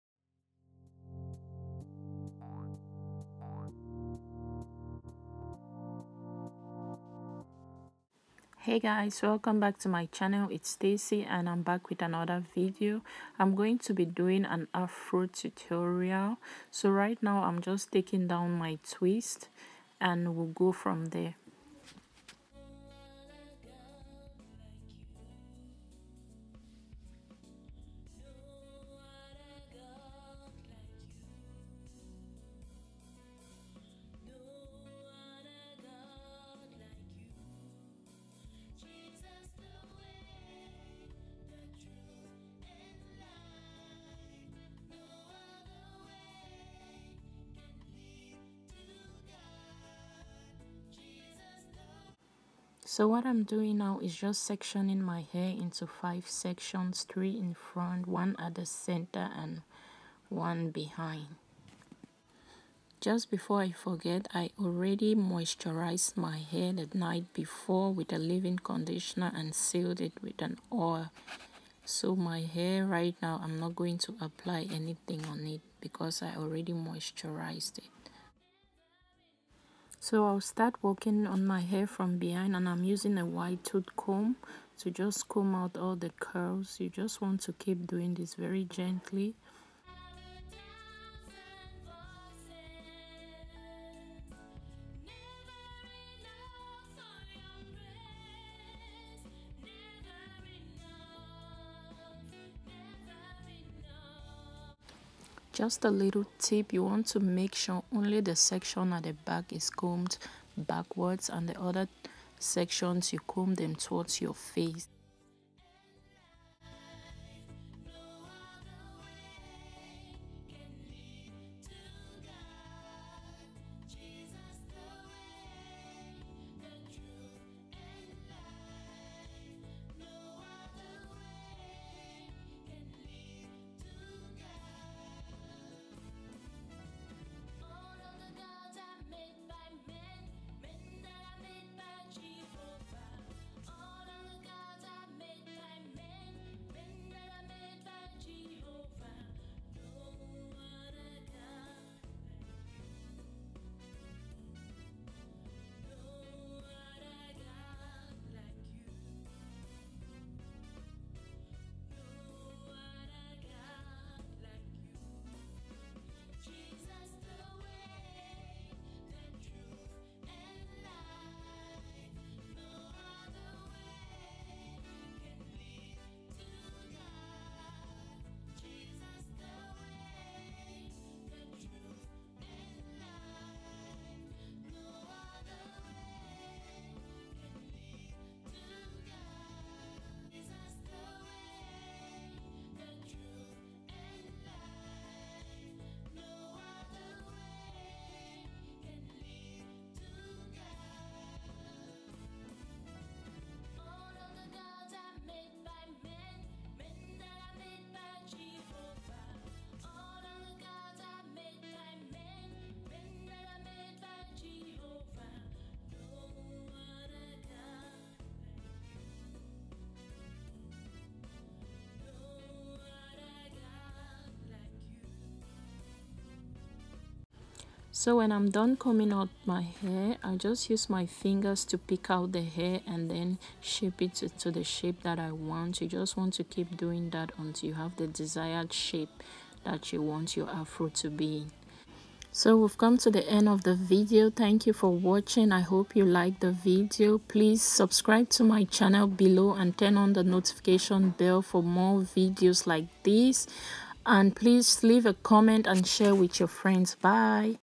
How I Get My Afro look | Natural Hair
In this video I share step by step how I get my natural hair into a perfect afro.It is quite easy and requires less time.